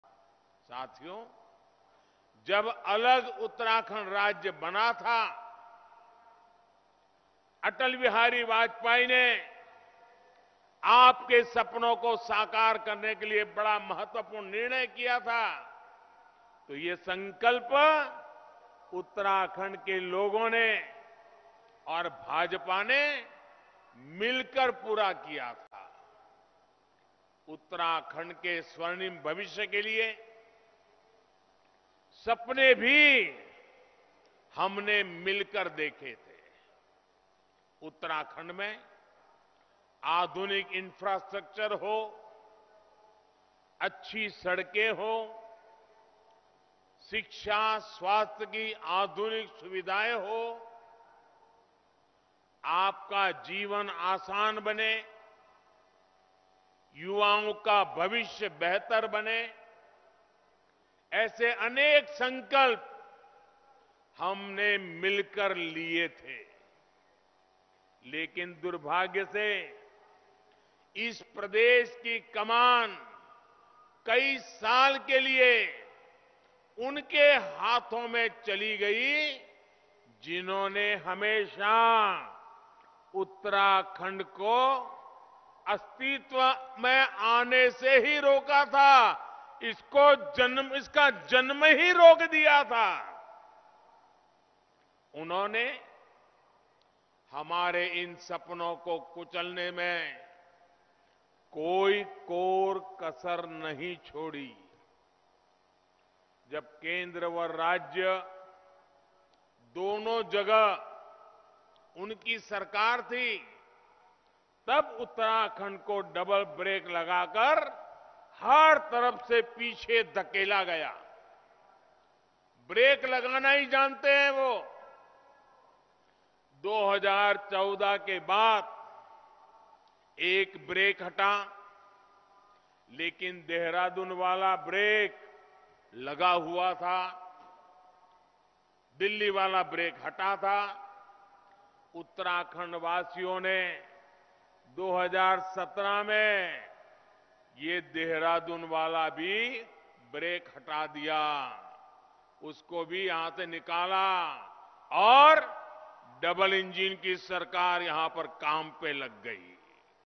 By electing double engine government, people of Uttarakhand removed all brakes on development: PM
Prime Minister Narendra Modi today addressed a public meeting in Srinagar, Uttarakhand. PM Modi started his address by reiterating his connection with Uttarakhand. “People of Uttarakhand know my connection and my love for the ‘Devbhoomi’ of this state,” he said.